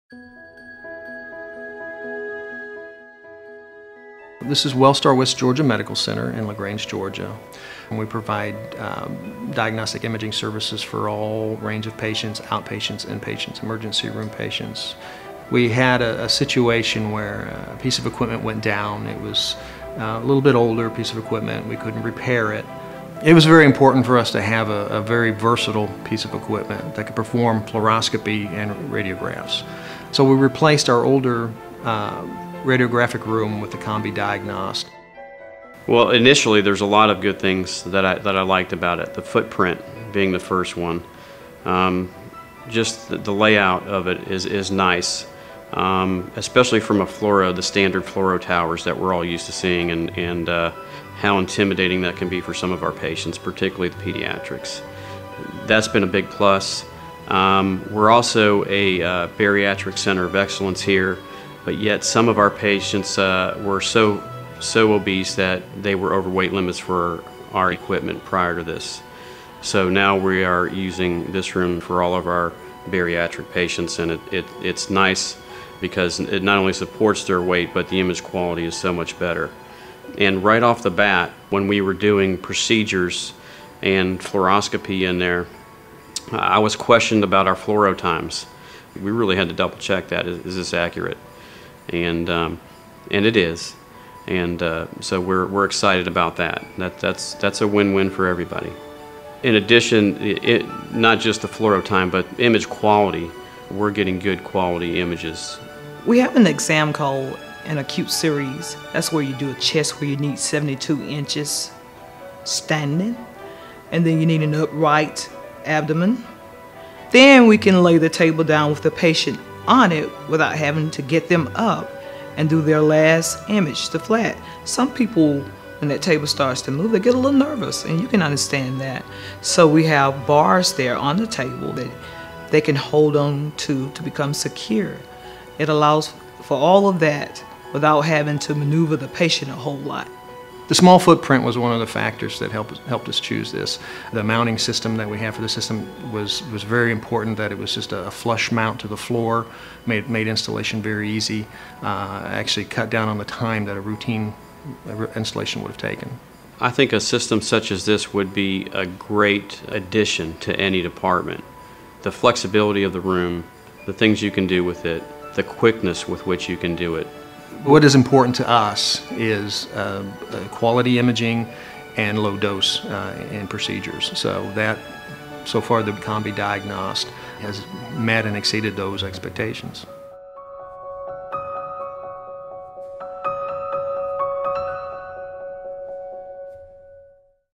Versatile radiography in a small space: CombiDiagnost R90
See how the versatility of the Philips CombiDiagnost is working for Wellstar in Western Georgia